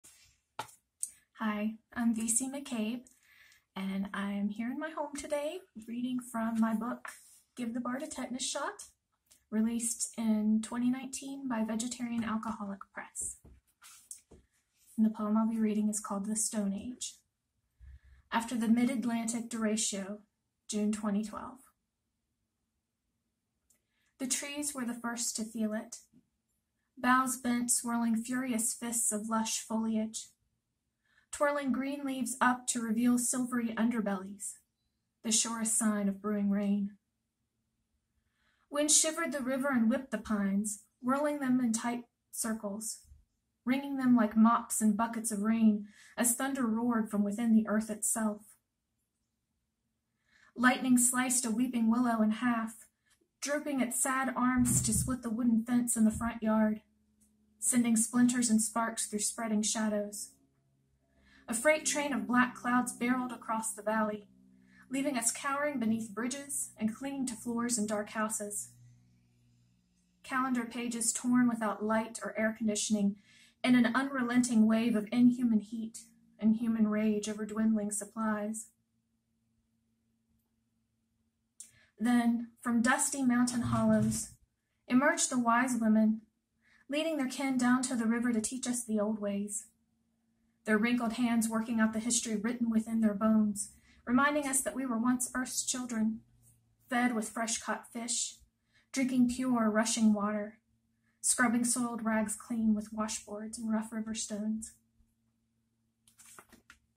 "The Stone Age" - V.C. McCabe
V.C. reads from 'Give the Bard a Tetanus Shot'